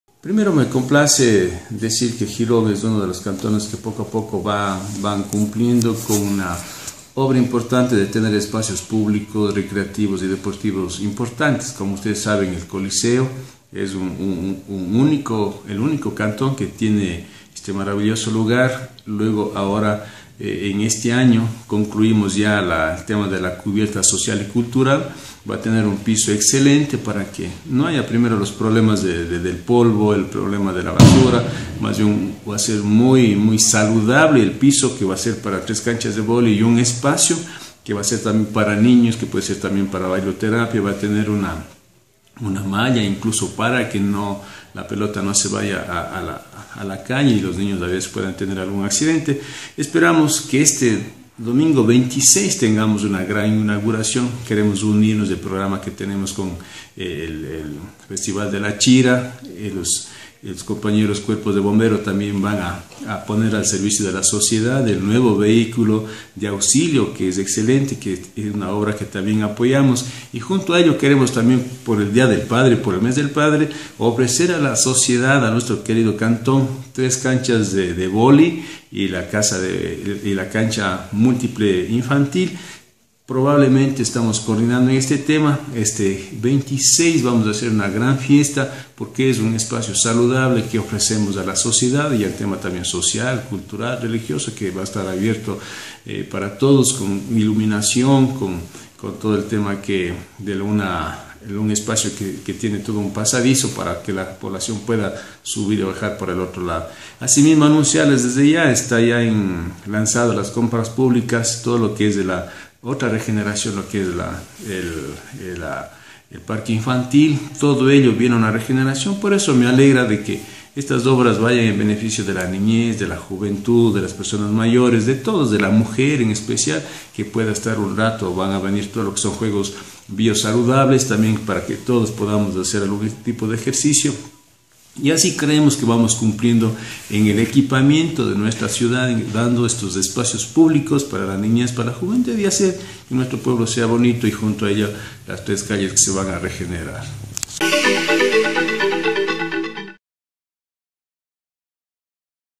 Anuncian inauguración de cubierta sobre canchas frente al CCG
Para el domingo 26 de junio, el GAD municipal de Girón tienen previsto la inauguración del techo sobre las conchas junto al parque infantil, cuyos trabajos están por concluir completamente a un costo de 286.424,87 dólares (según consta en el letrero colocado en el atrio del salón del pueblo), paralelo a este acto, se desarrollará el IV Festival de la Achira, así lo dio a conocer el alcalde José Miguel Uzhca.